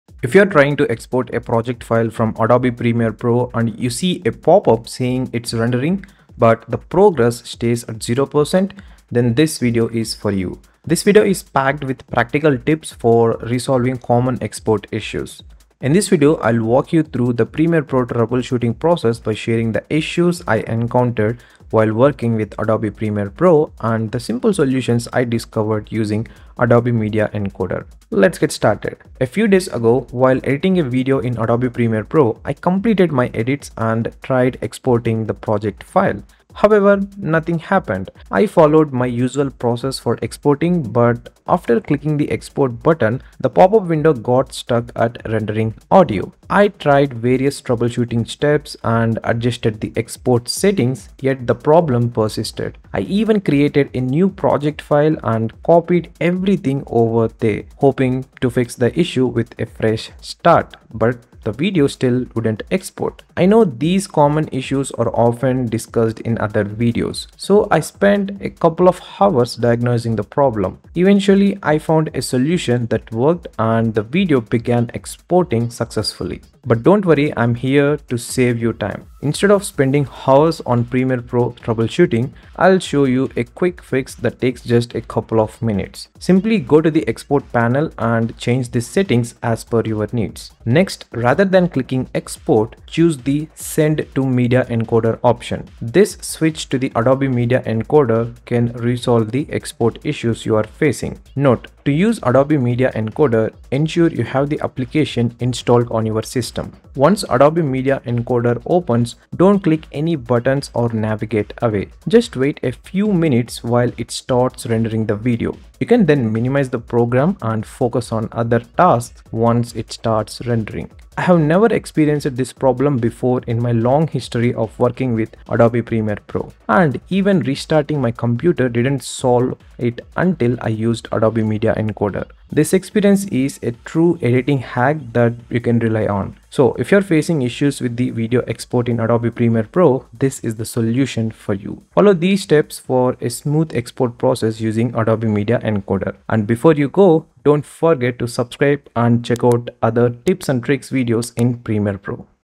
Fix Adobe Premiere Pro Export Stuck at Rendering Required Audio Files
In this video, I explain how to quickly resolve Adobe Premiere Pro export issues when your video gets stuck at 0% rendering at required audio files. This tutorial is a must-watch for anyone facing export problems in Premiere Pro.